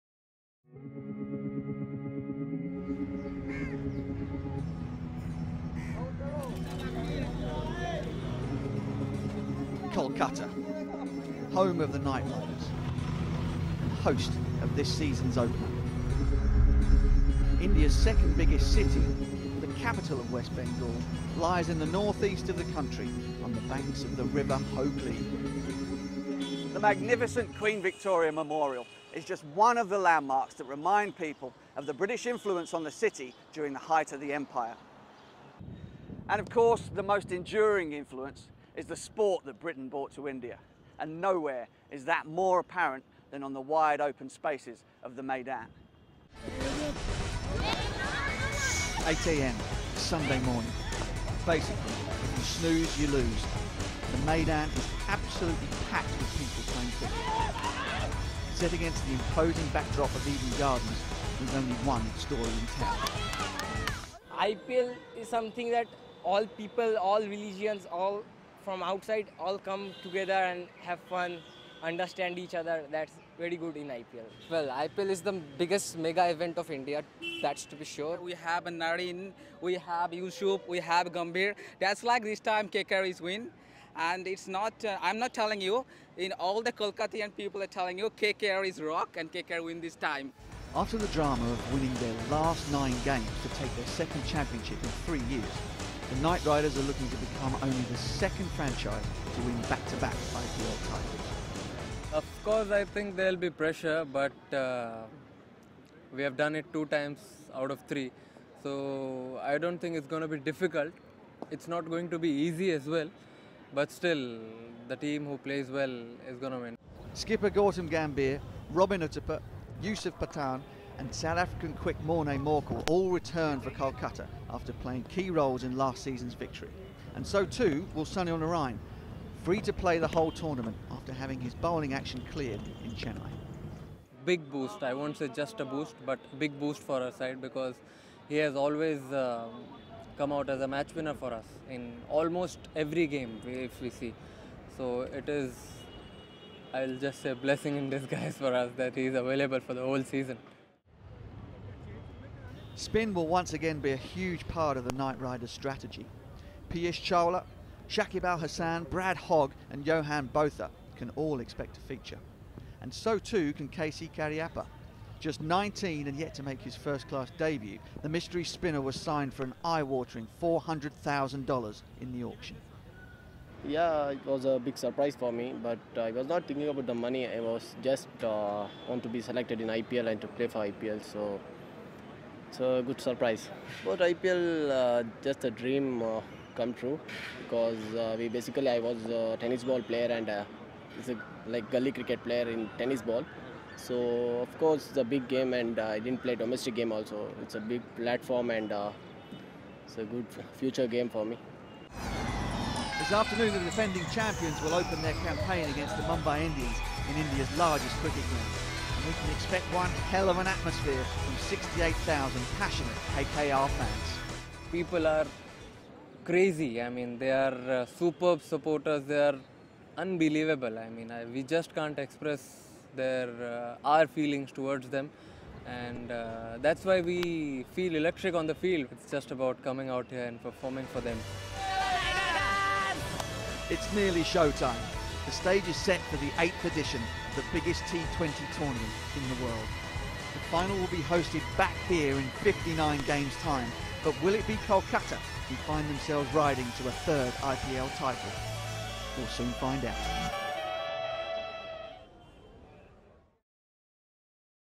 SKY SPORTS PREVIEW OF KKR WITH MARK BUTCHER
One of the world's biggest sports broadcasters aired a preview of KKR in IPL 2015 with former English player Mark Butcher.